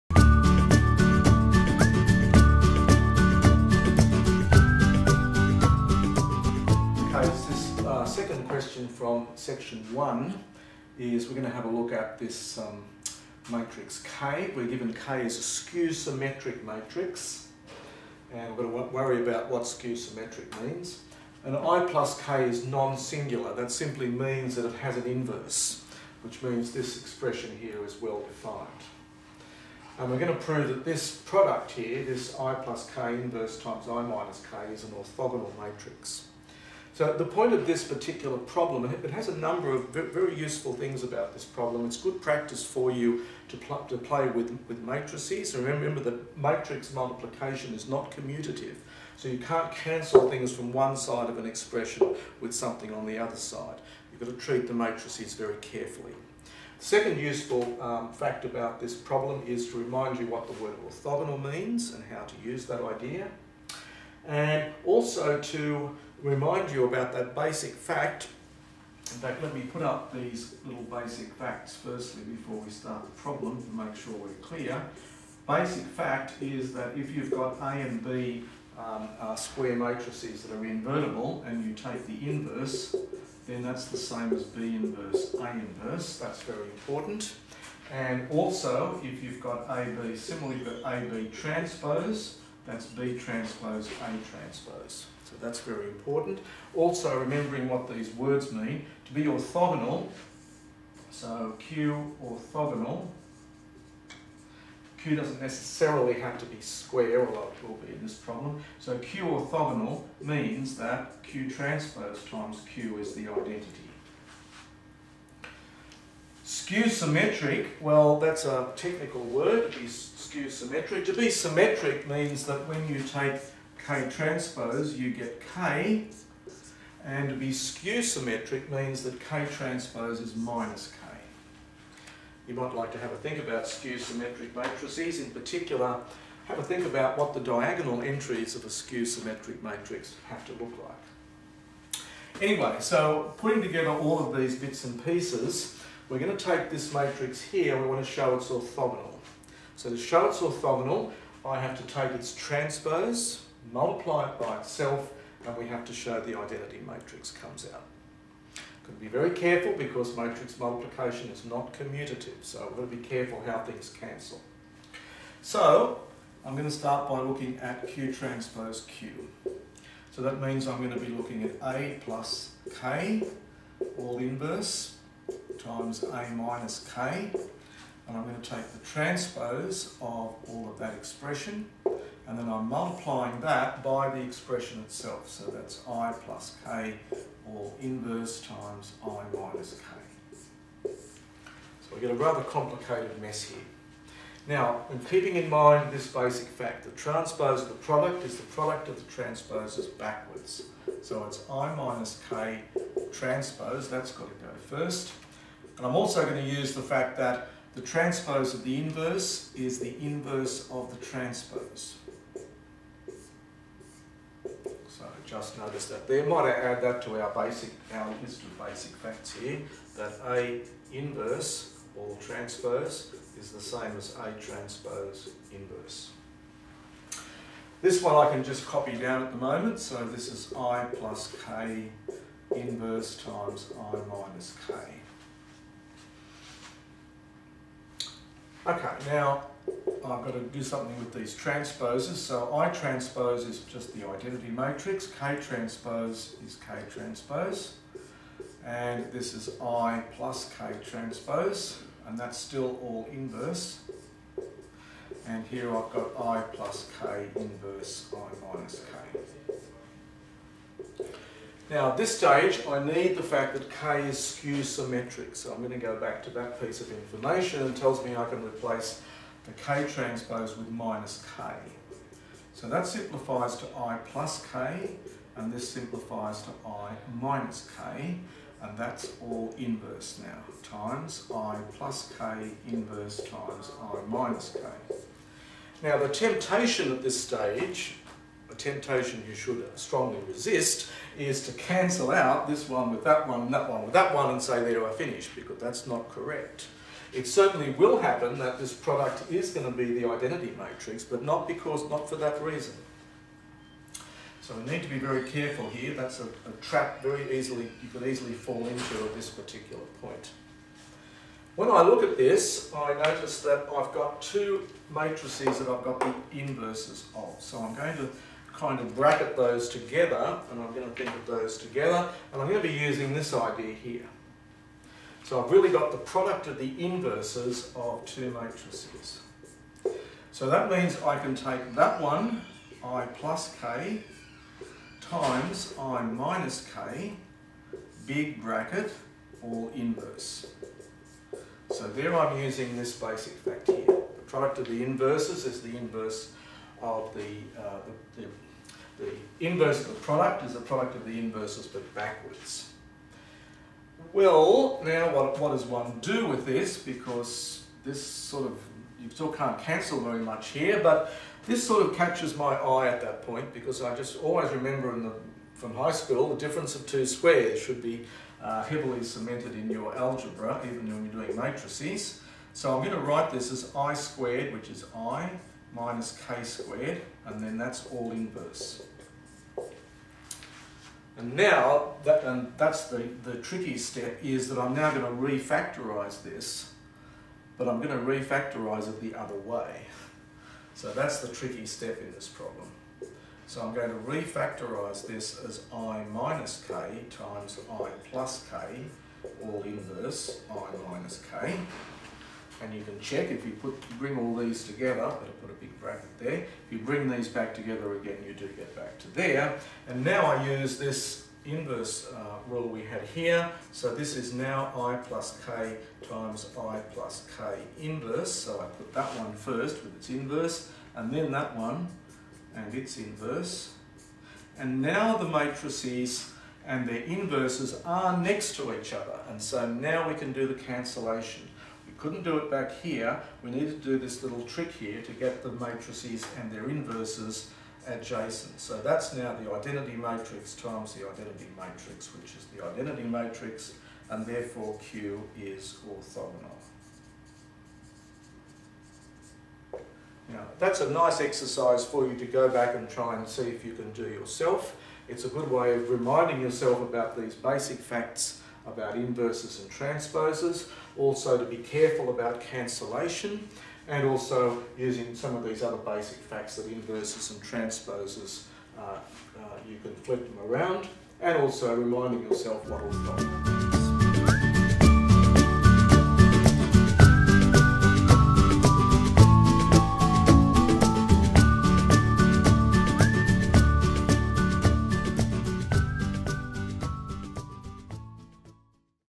Linear Algebra 01: Prove a Matrix is Orthogonal (Ch1 Pr2)
This is Chapter 1 Problem 2 from MATH2501. Presented by Peter Brown.